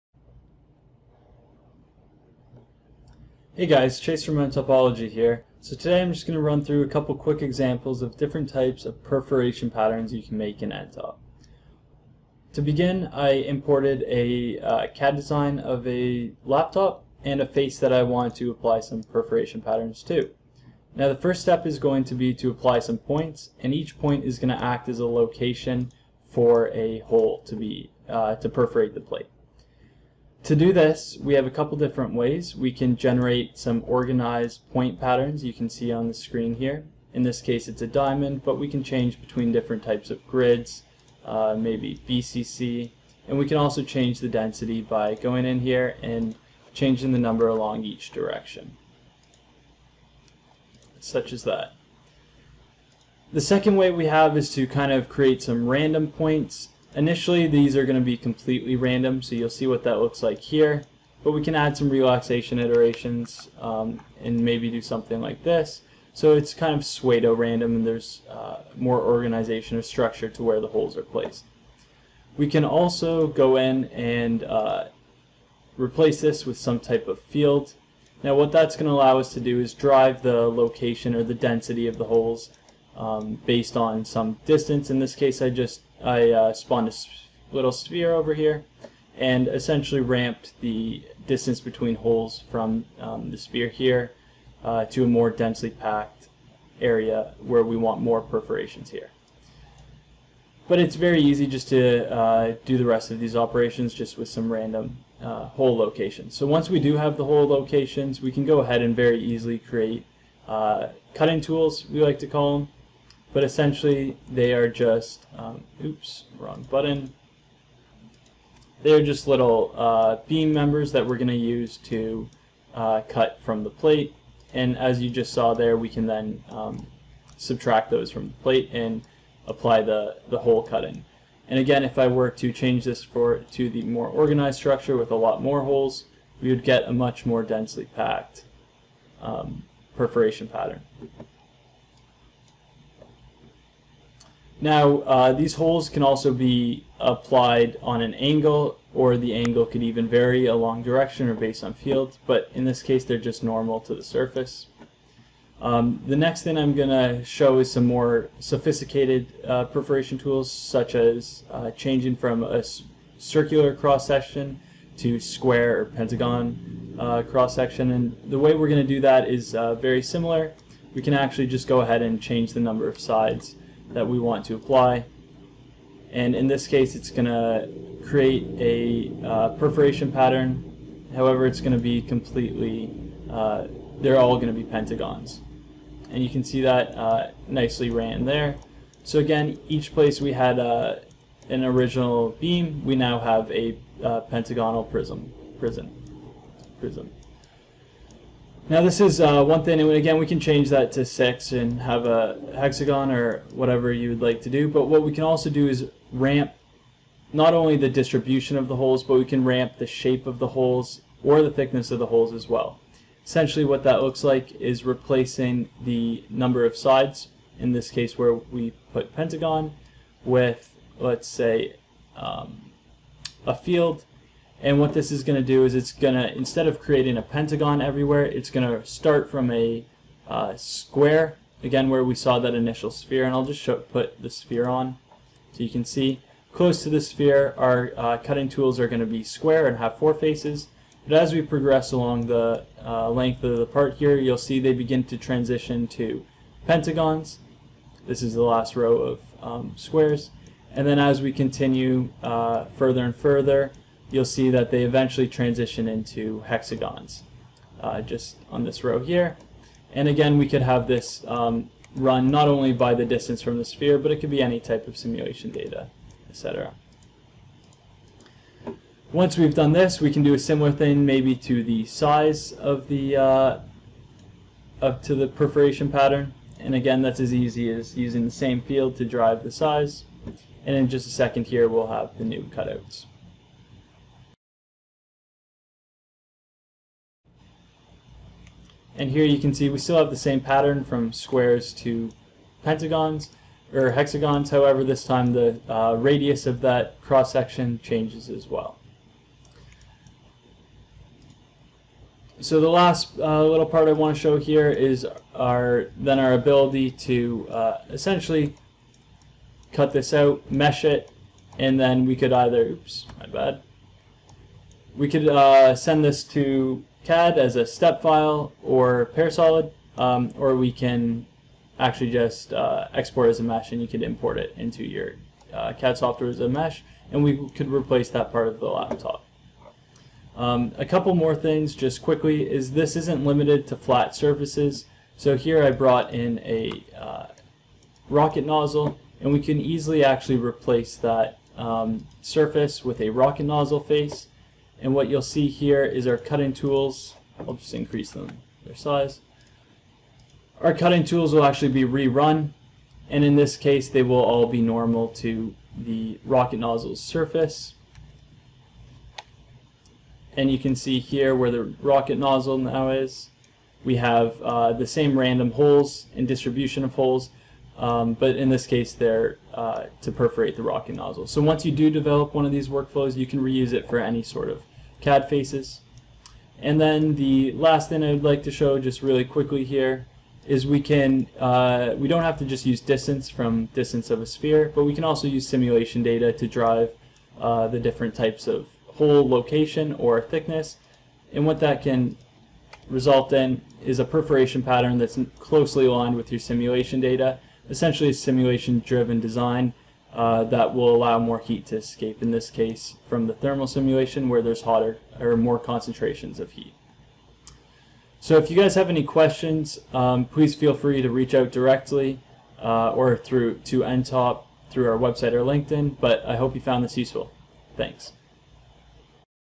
nTop Live: Automated Generation of Complex Hole & Perforation Patterns
Perforation patterns are extremely common in engineering. They are used for ventilation, thermal management, lightweighting, or even as decorative elements. Yet, creating non-standard or conformal patterns is not a simple task in traditional CAD software.

In this nTop Live, Chase Allan, Solutions Engineer at nTopology, demonstrates clever ways to add complex perforation patterns to any part in nTop Platform using the available advanced latticing tools.

Watch and learn how to:
- Use latticing workflows to create complex perforation patterns
- Create cutouts with a non-round shape or non-uniform size
- Use fields to spatially control the size and location of the perforations

Don’t have a license of nTop Platform yet?